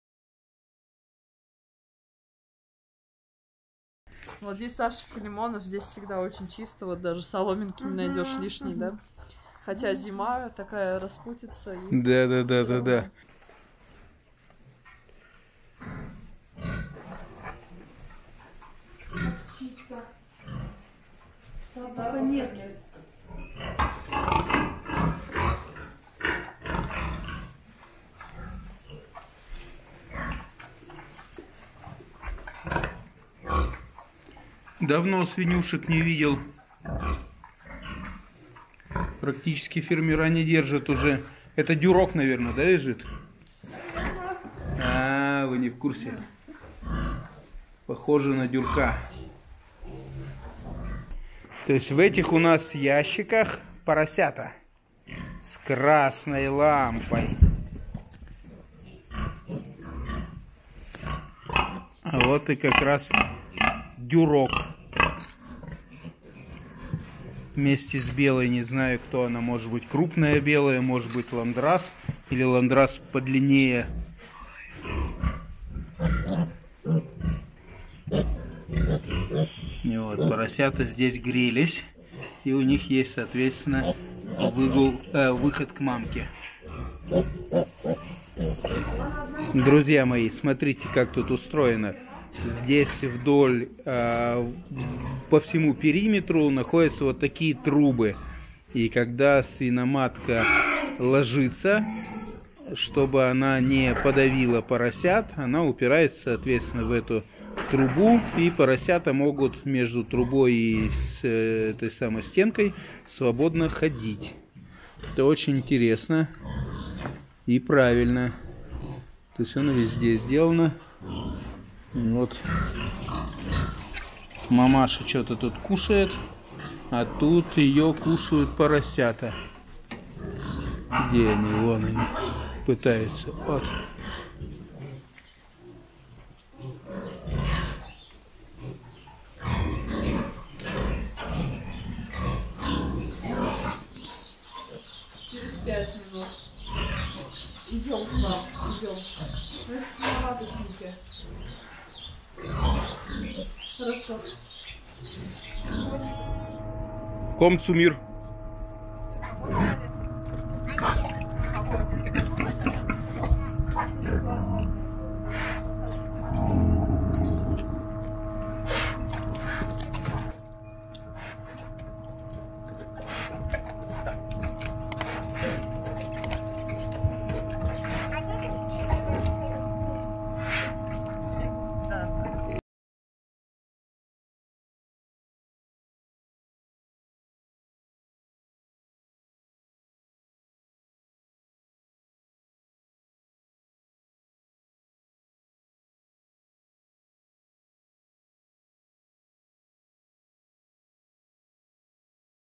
Разведение свиней. Свиноматки, поросята. Ферма Лукино
Разведение свиней в КФХ Лукино. Тульская область. Февраль 2020 г.
ЖМИ 👇"ЕЩЁ"👇 КОНТАКТЫ ТАМ!
Отлично работающий сельскохозяйственный бизнес. Особенно было приятно, что десять лет назад всю информацию Евгений брал на нашем портале Поэтому друзья, читайте, изучайте, задавайте вопросы на портале!